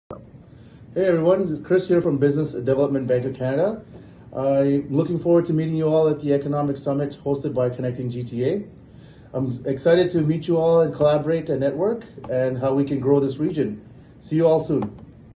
Kris Poopalasingam Senior Account Manager Business Development Bank of Canada
Kris Poopalasingam, Senior Account Manager at the Business Development Bank of Canada, urges you to join the summit. Don't miss out on the chance to be part of shaping Ontario's economic future. Let's collaborate and unlock new opportunities together!